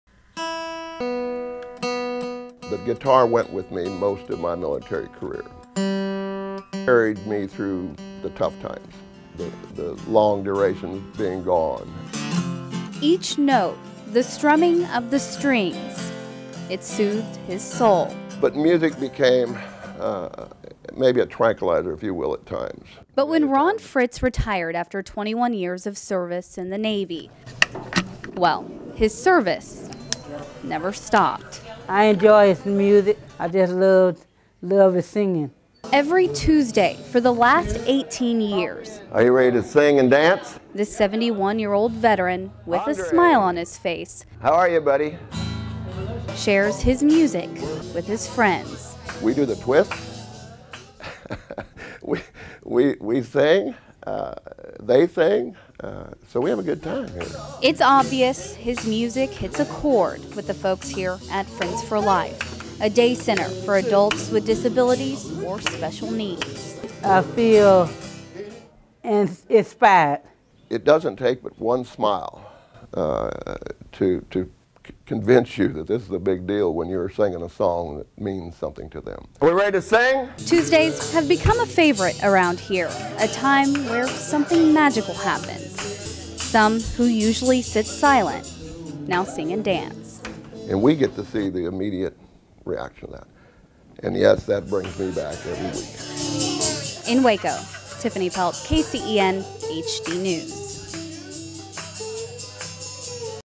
Veteran Gives Gift of Music to Heal Others
(This is one of my FAVORITE stories!) Navy Veteran Ron Fritz takes his guitar everywhere he goes. It's what helped get him through the long deployments. But now, at 71 years old, Ron shares that musical talent with others. For the last 18 years he's been playing music at Friends for Life, a day center for adults with disabilities and special needs. With each note, he's helping to heal others.